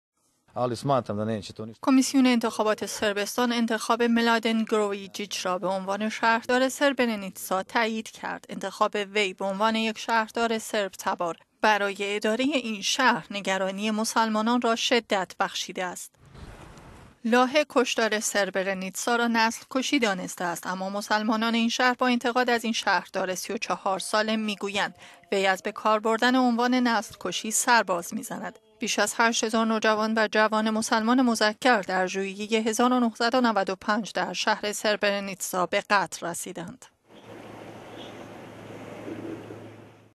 یک صرب تبار شهردار سربرنیتسا شد
کمیسیون انتخابات در صربستان انتخاب یک شهردار صرب تبار برای ادارۀ شهر سربرنیتسا را تایید کرد. در این انتخابات که روز دوم اکتبر برگزار شد «ملادن گروییچیچ» که یک صرب ملی گراست بعنوان شهردار این شهر برگزیده شد. انتخاب وی نگرانی مسلمانان این منطقه را شدت بخشیده است.

دیوان بین المللی لاهه کشتار سربرنیتسا را نسل کشی دانسته است اما مسلمانان سربرنیتسا با انتقاد از این شهردار ۳۴ ساله می گویند وی از بکار بردن عنوان نسل کشی سرباز می زند.

شهر سربرنیتسا که صحنۀ بزرگترین قتل عام در اروپا بعد زمان جنگ جهانی د…